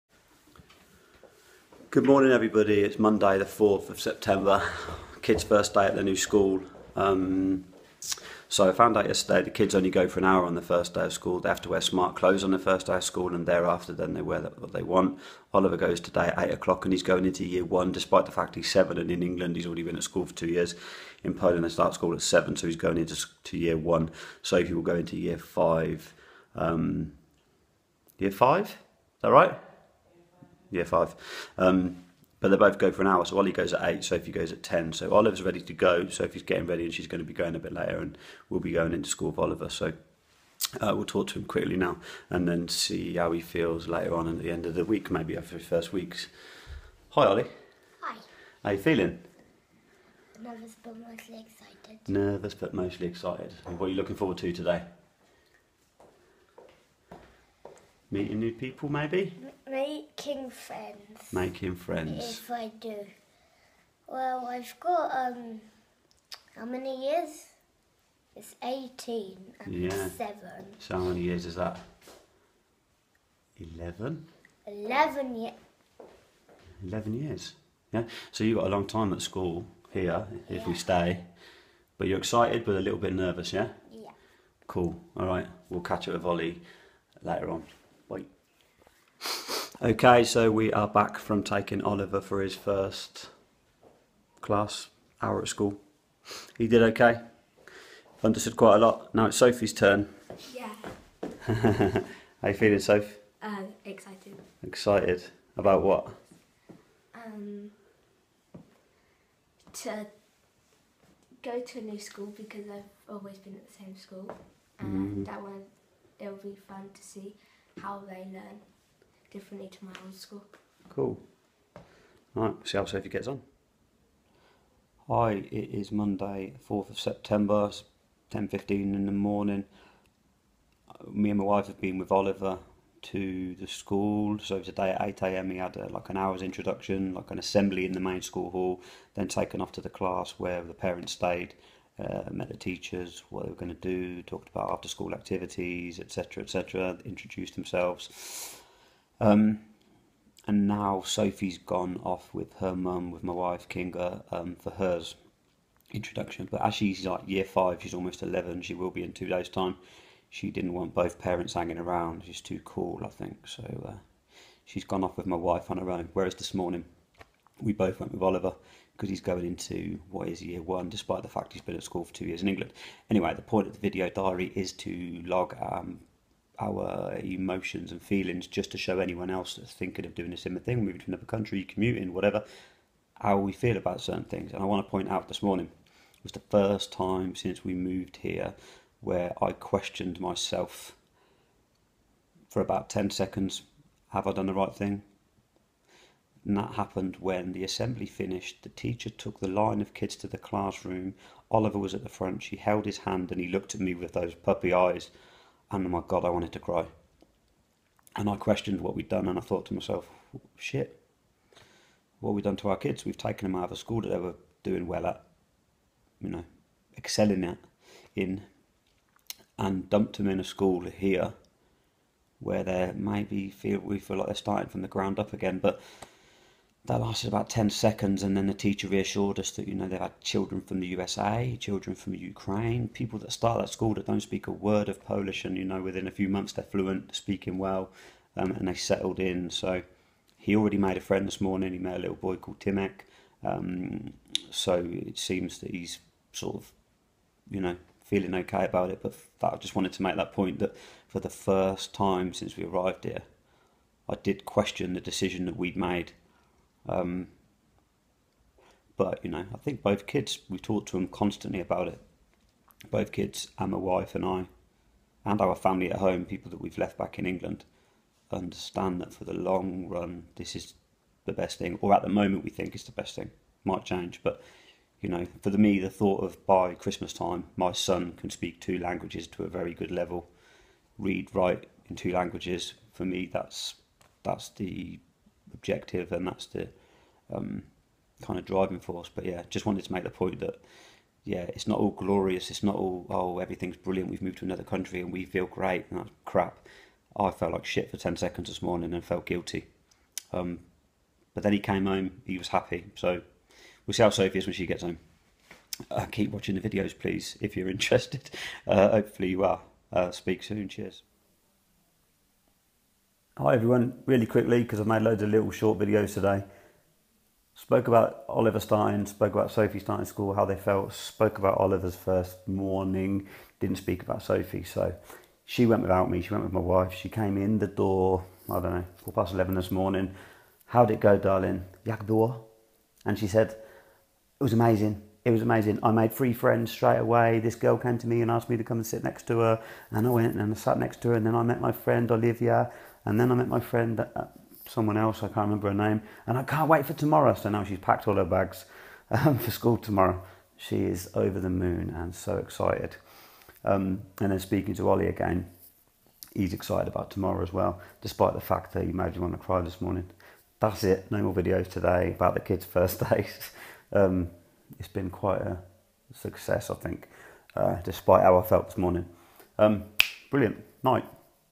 The Kids first day at a Polish school in Wroclaw. (The Super Commuting Dad)
How the first day went for my two kids (Sophie aged 11 and Oliver aged 7) at a School in Wroclaw, Poland. They have always been to school in the UK up until July 2017. My daughter already speaks Polish very well and my son understands. He is very reluctant to speak it though so we hope this experience will give him some confidence.